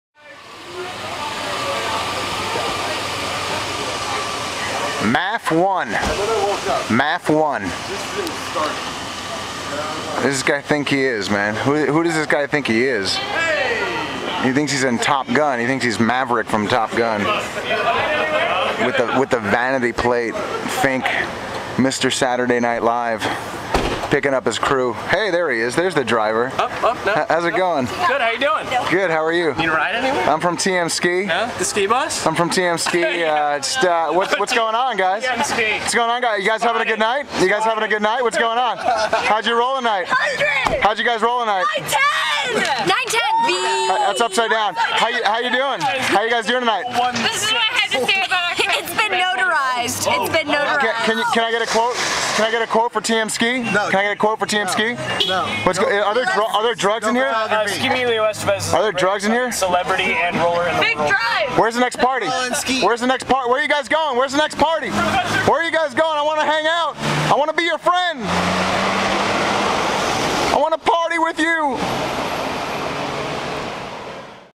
TMSkee
TMSkee - Fink driving the Skee Bus late on Monday night. Skeemelio Estevez and Punkskee Brewster were off to an undisclosed late-night party.